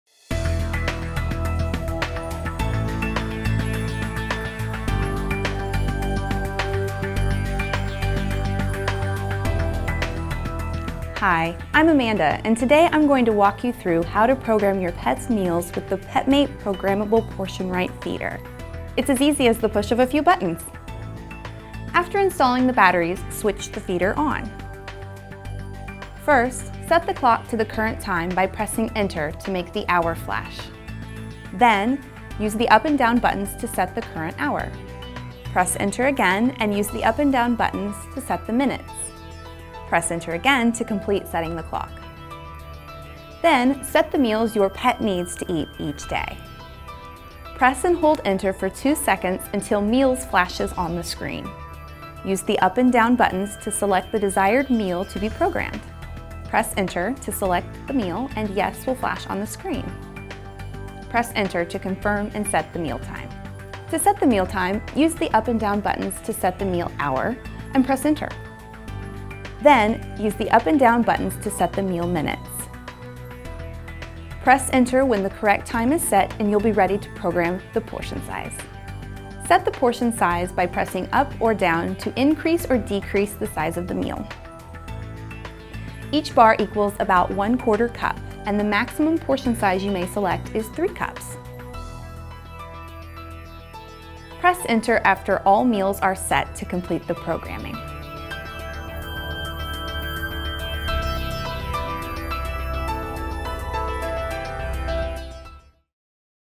How to Program Your Petmate Programmable Portion Right(R) Feeder
Programming your Petmate Programmable Portion Right Feeder is as easy as the push of a few buttons. Watch to learn more!